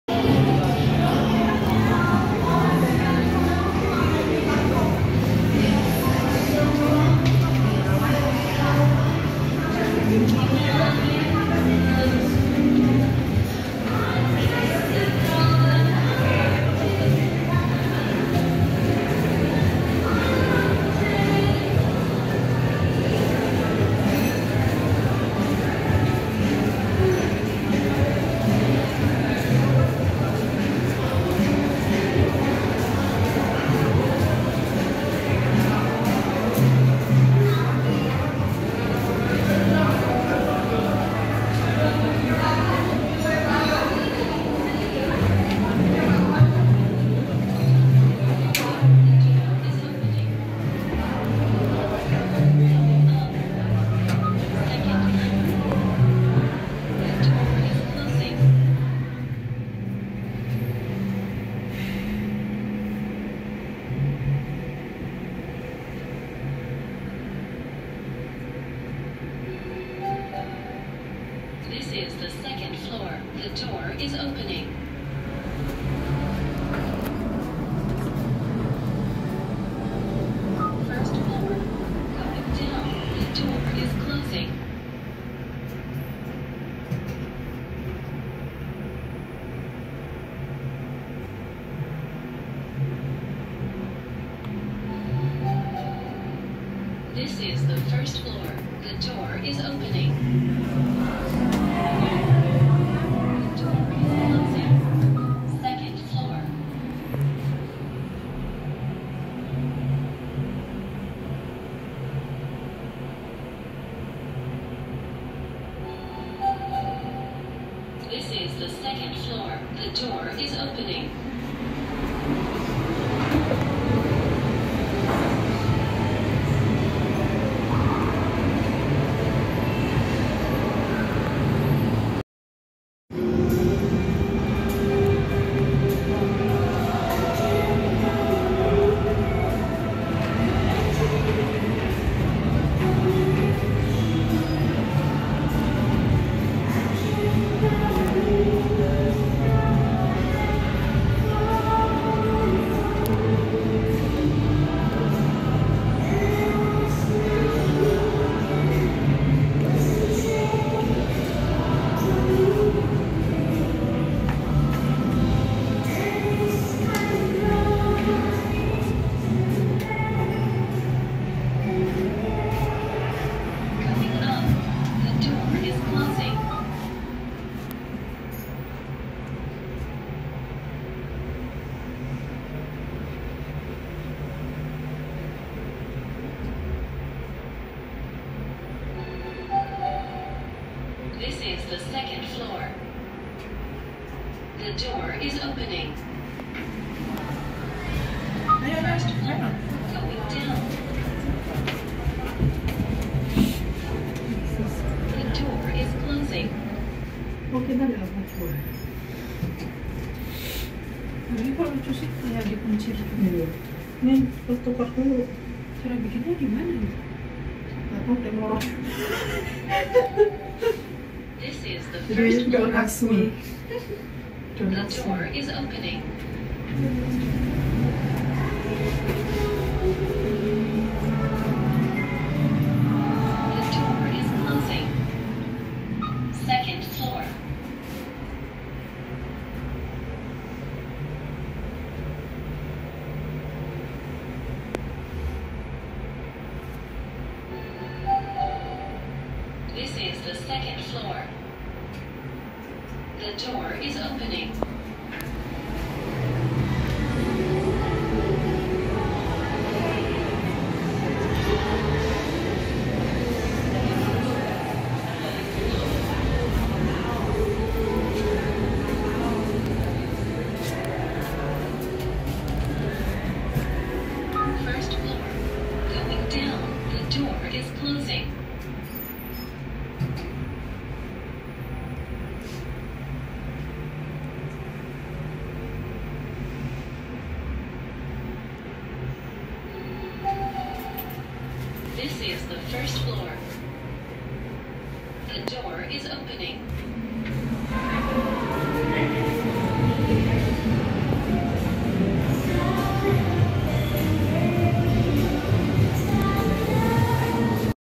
2023 Hyundai WBSS 2 MRL Elevators at Gafoy Kelapa Gading, Jakarta
There are 6 accessible elevators here, but only 2 elevators that operating right now. The capacity is quite small, so not many poeople can use this elevator at the same time. The COP indicator is LCD like the one at Roxy Square and there is also talkative voiceover. From the HOP, this elevator should have Touchless Foot Button.

Building Information
Name: Gafoy
Address: Boulevard Raya Rd, Kelapa Gading, North Jakarta, Jakarta 14240, Indonesia

Elevator Information
Brand: Hyundai
Model: WBSS 2
Fixtures: Type 64 (with Touchless Foot Button)
Year of installed: 2023
Speed: 1 m/s
Installed by: PT. Hyundai Elevator Indonesia
Floor Service: 1, 2 (2 floors)
Capacity: 800 kg / 12 persons
History: Original
Filming Difficulty: Easy

Camera Information
Device: Samsung Galaxy A52
Resolution: 1080p | 30 FPS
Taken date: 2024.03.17

Please ask permission first to reuse or reupload my video. Any misuse or unauthorized action will be reported.